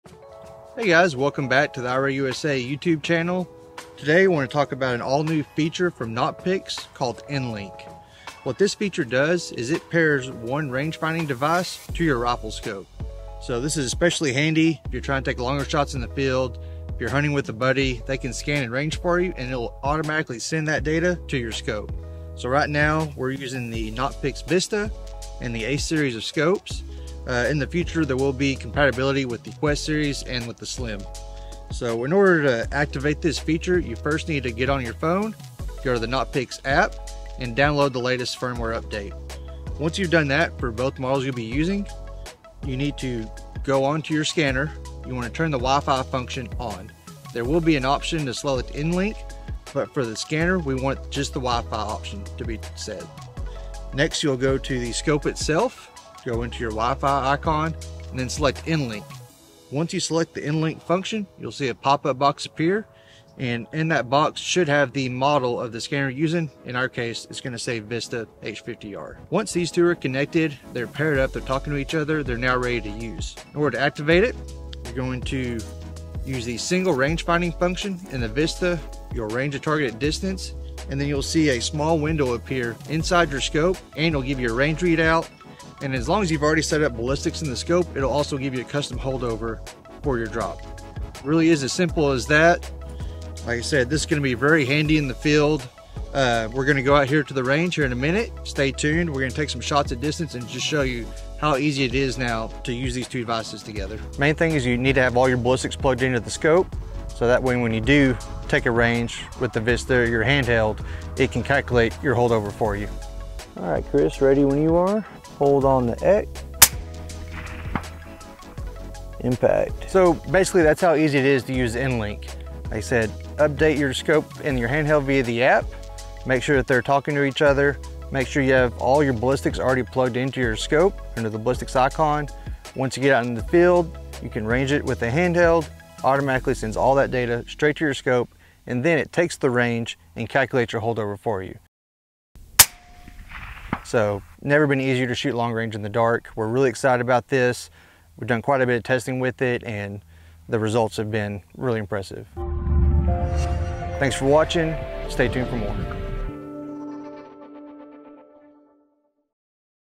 New N-Link Software from Nocpix, Check it out!
The all new N-Link function from Nocpix allows you to pair a range finding monocular with your scope. For example, a VISTA H50R can connect to an ACE L35 to give you real time range and ballistics data directly on the scope's screen. Shooting long range with a thermal has never been easier!